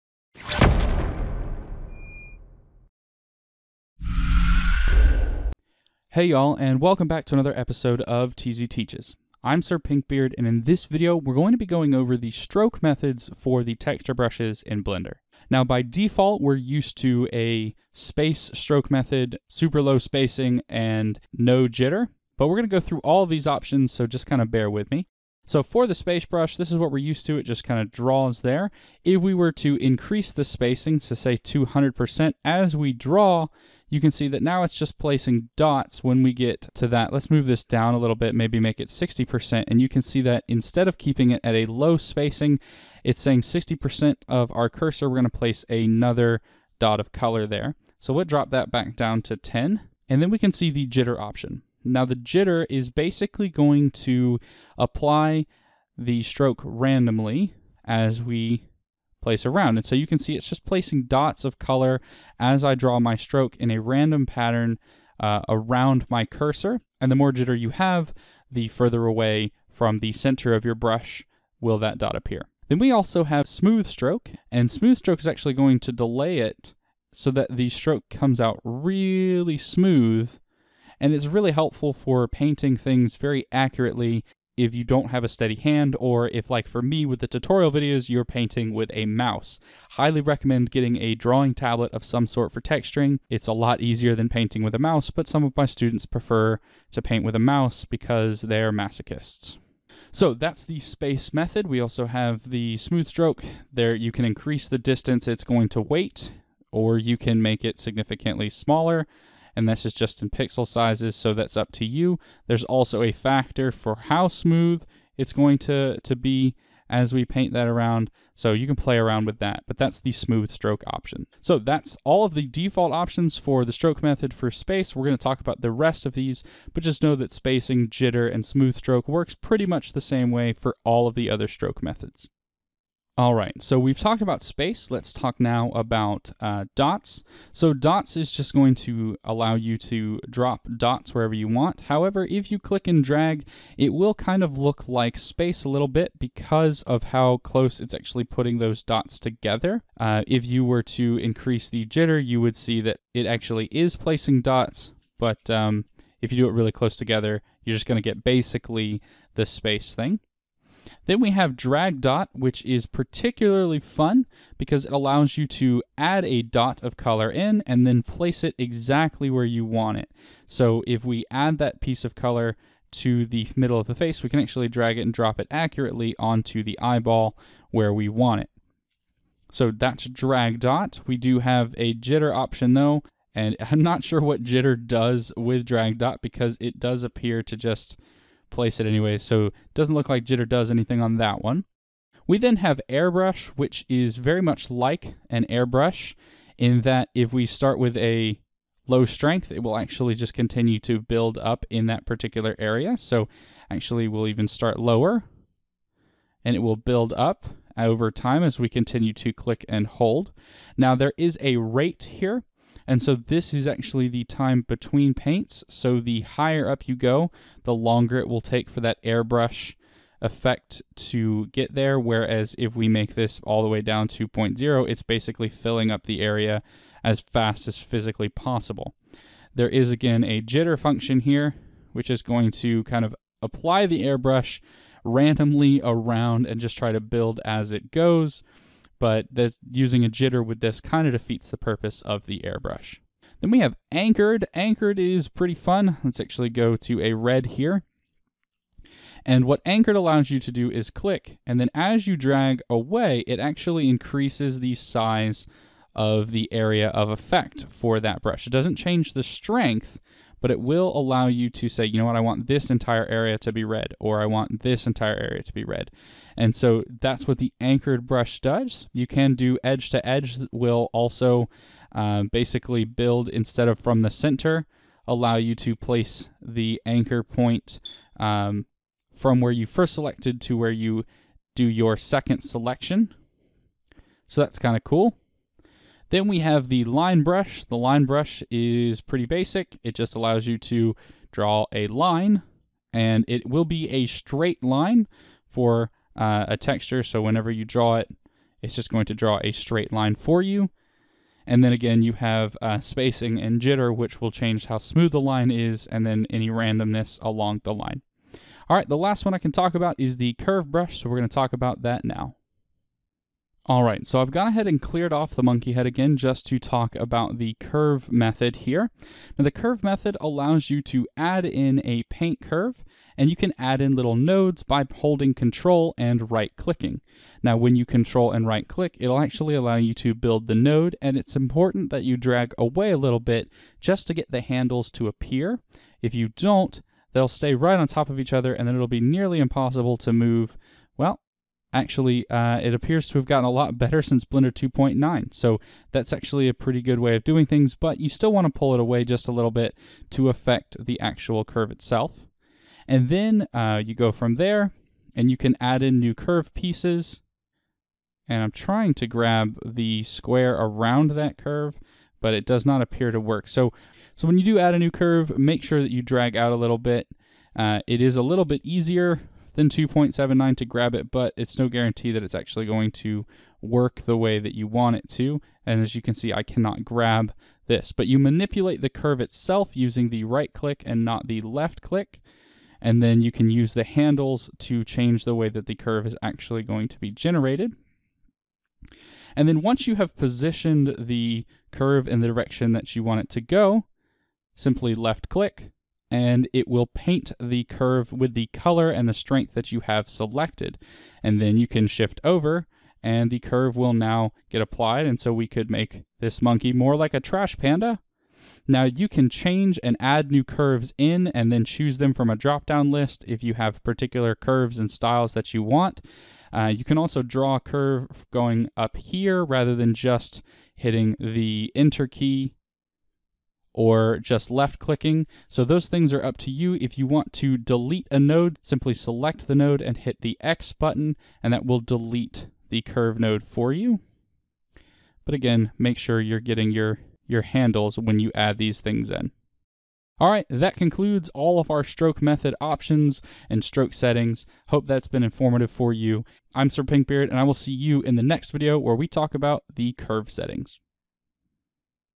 Stroke Methods for Texturing in Blender 2.8
This video is the 9th of 11 videos that make up the Texturing in Blender 2.8 video series. It covers how to adjust the different stroke methods to paint in the way you want. Changing these settings allows full control over how the paint will be applied to your model.

This course covers the basics of texturing in Blender 2.8 and some tips and tricks to make texturing your models even easier. It is really meant for new Blender users, but if you’re transitioning from 2.79, this course could show you a couple of things that are new to using 2.8.